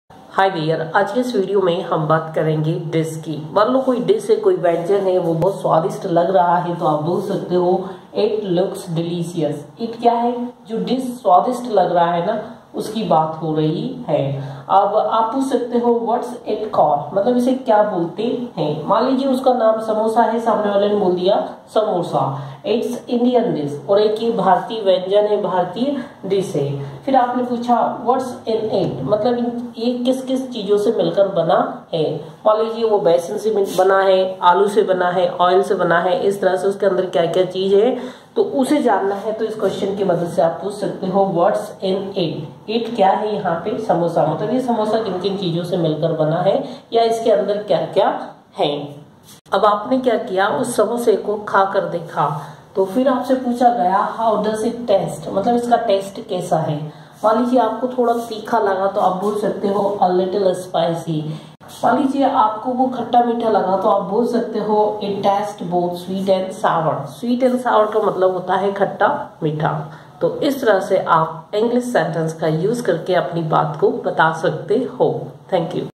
Sweet and sour meaning in hindi || A little spicy meaning in hindi || What's in it means
Sweet and Sour का हिंदी क्या होता है।

साथ ही और देखें Dish से related English sentences और उनके हिंदी मीनिंग।

If you want to improve your English language knowledge.